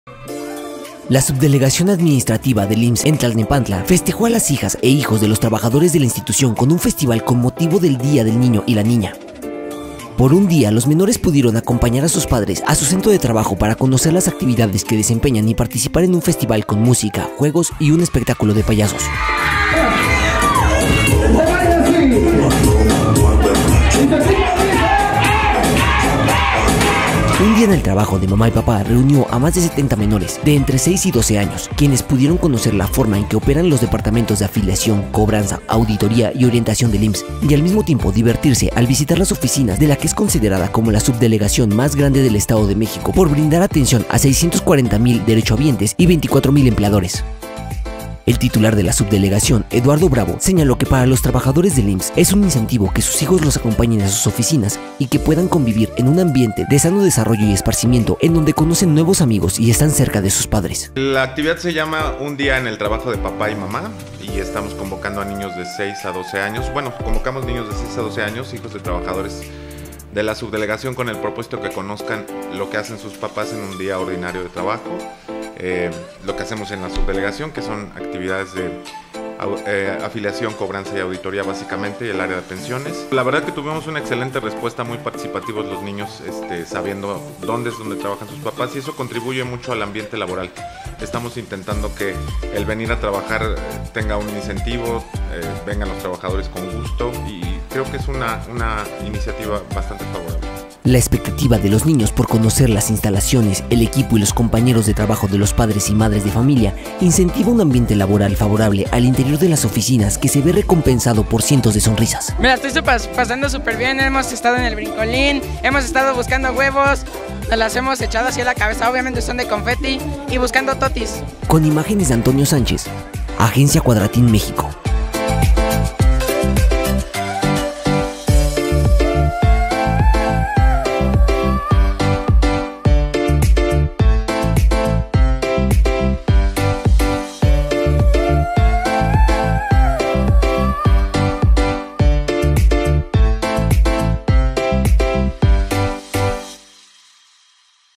Sonríen niños previo a su día en IMSS-Tlalnepantla
CIUDAD DE MÉXICO, 29 de abril de 2022.- La subdelegación administrativa del Instituto Mexicano del Seguro Social (IMSS) en Tlalnepantla festejó a las hijas e hijos de sus trabajadores con un festival con motivo del Día del Niño y la Niña.
Por un día, los menores pudieron acompañar a sus padres a su centro de trabajo para conocer las actividades que desempeñan y participar de un festival con música, juegos y un espectáculo de payasos.
Un día en el trabajo de mamá y papá reunió a más de 70 menores, de entre seis y 12 años, quienes pudieron conocer la forma en que operan los departamentos de Afiliación, Cobranza, Auditoría, y orientación del IMSS, y al mismo tiempo divertirse al visitar las oficinas de la que es considerada como la subdelegación más grande del Estado de México, por brindar atención a 640 mil derechohabientes y 24 mil empleadores.
El titular de la subdelegación, Edgar Bravo destacó que para los trabajadores del IMSS es un incentivo que sus hijos los acompañen a sus oficinas y que puedan convivir en un ambiente de sano desarrollo y esparcimiento en donde conocen nuevos amigos y están cerca de sus padres.
El directivo consideró que los festejos por el Día de la Niña y el Niño fomentan la convivencia de igualdad entre menores, pero también los padres pueden explicarle a sus hijos cuáles son sus funciones cotidianas y la importancia que tiene su trabajo para el funcionamiento de la institución pública, cuya área administrativa está conformada por 480 trabajadores.
La expectativa de los niños por conocer las instalaciones, el equipo y los compañeros de trabajo de los padres y madres de familia, incentiva un ambiente laboral favorable al interior de las oficinas que se ve recompensado por cientos de sonrisas.